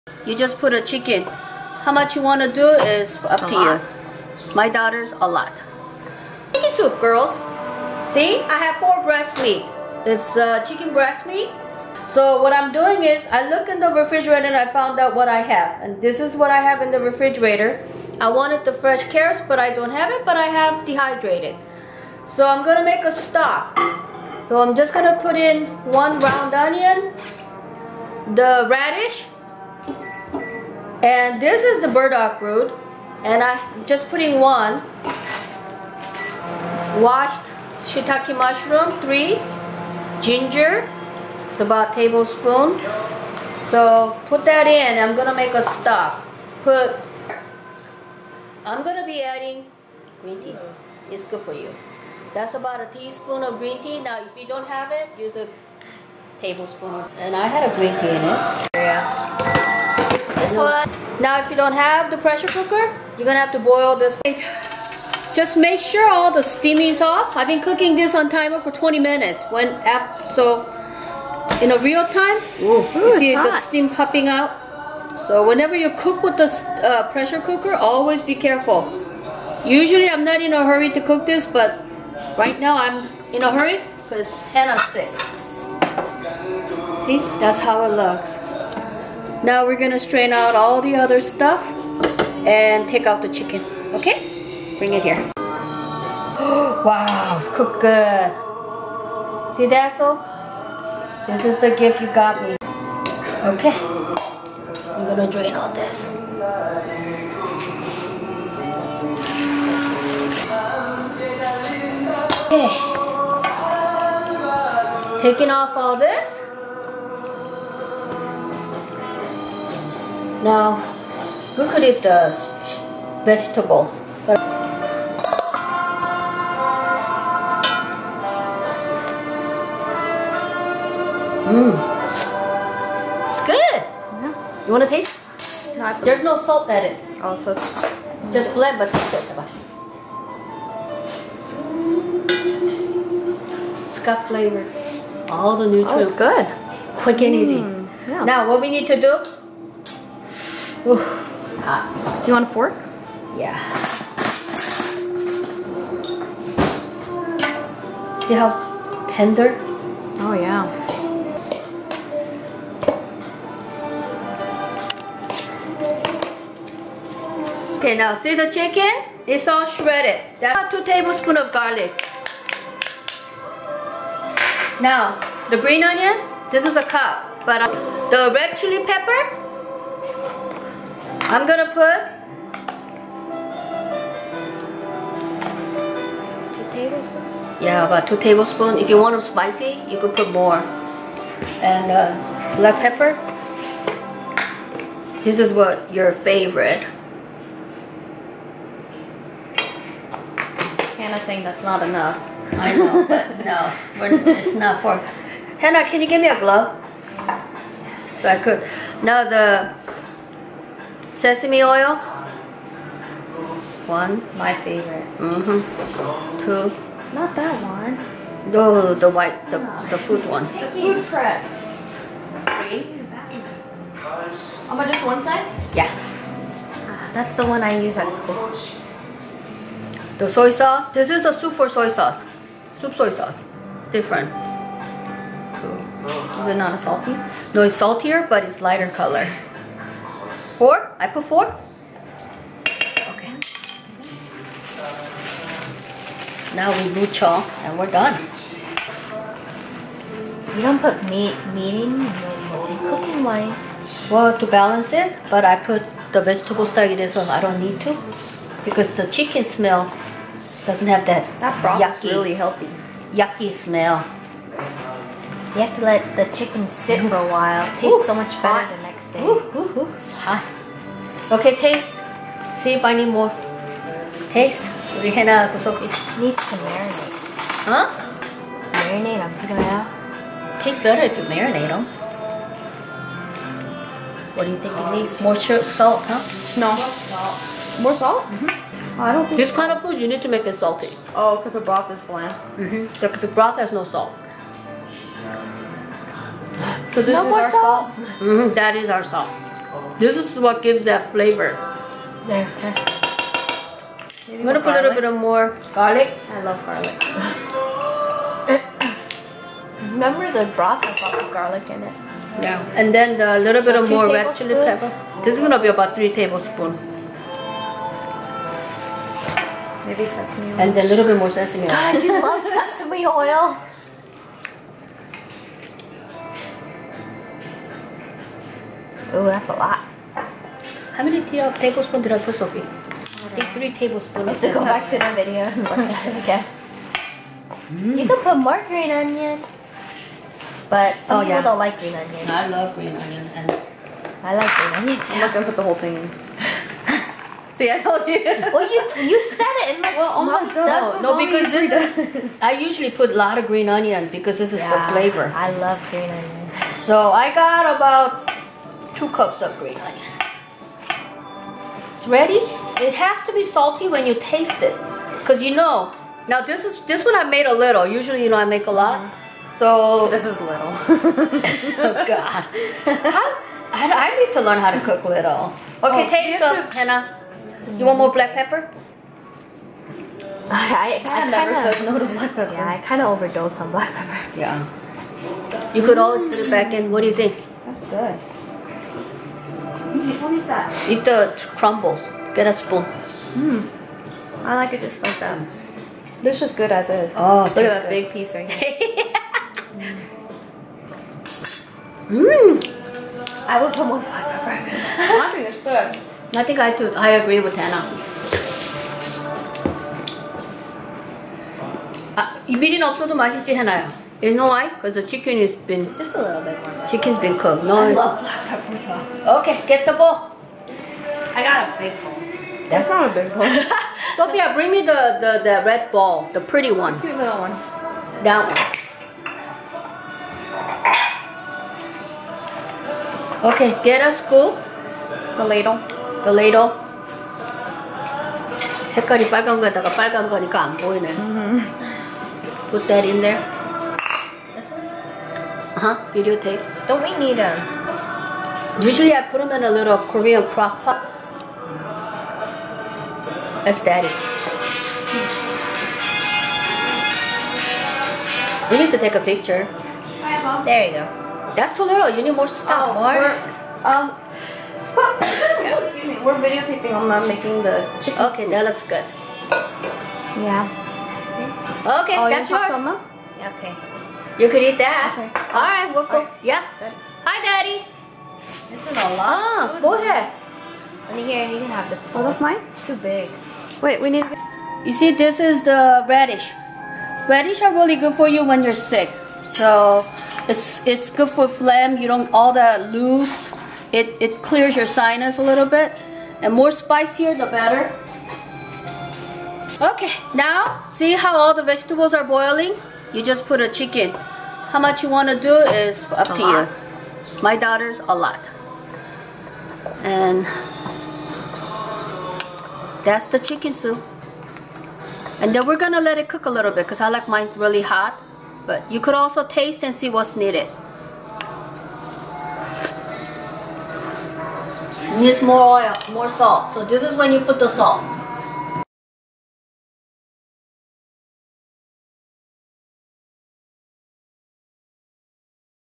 Vlog: Cooking Healthy Spicy Chicken Soup with my girls by Omma's Home
Spicy Chicken Soup (닭국) Home remedy for winter colds for my girls by Omma’s Home

Serving Portion: 4-6 people

Ingredients:

4 chicken breast meat
1 round onion (any size will do)
1 small radish or ab